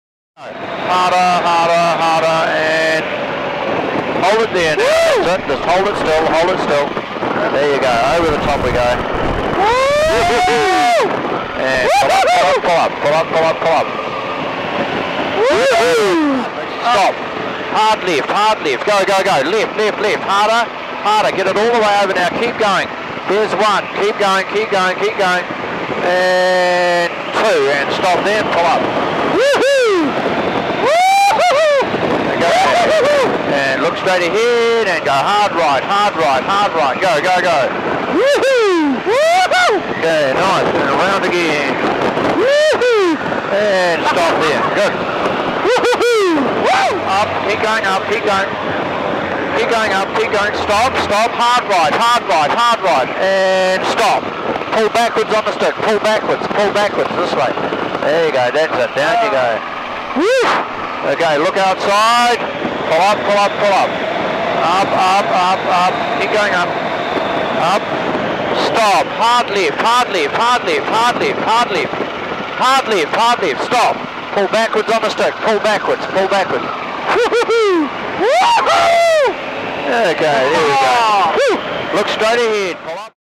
Ursl flying a stunt plane with
Ursl flying a Pitts Special over the Abel Tasman National Park in New Zealand and doing aerobatics